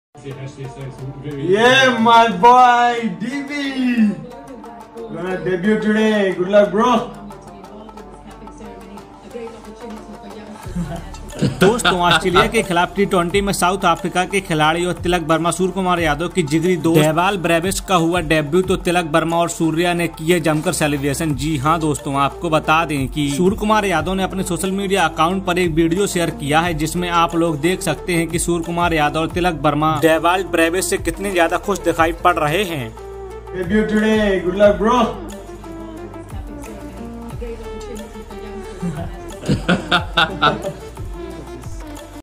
Surya & Tilak Verma Crazy Celebration On Dewald Bravis Debute In SA vs AUS T20 Series
Surya & Tilak Verma Crazy Celebration On Dewald Bravis Debute In SA vs AUS T20 Seri...